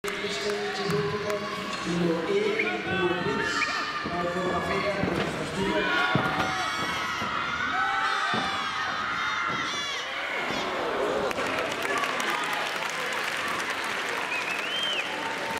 Alex Renkert - Pass 1 - Tumbling - 2015 World T&T Championships Qualifying
Nov. 26, 2015 - Stadium Arena Fyn - Odense, Denmark
2015 Trampoline & Tumbling World Championships